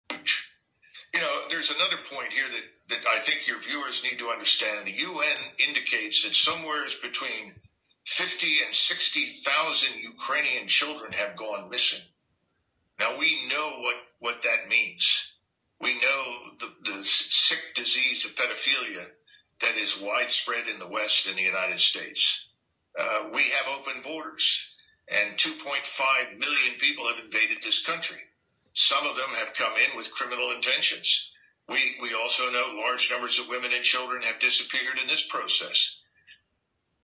Τα «αγνοούμενα» 50-60 χιλιάδες παιδιά της Ουκρανίας έχουν γίνει λεία των δυτικών παιδεραστών
Σύμφωνα με τον απόστρατο συνταγματάρχη του αμερικανικού στρατού Ντάγκλας ΜακΓκρέγκορ, τα «αγνοούμενα» 50-60 χιλιάδες παιδιά της Ουκρανίας έχουν γίνει λεία των δυτικών παιδεραστών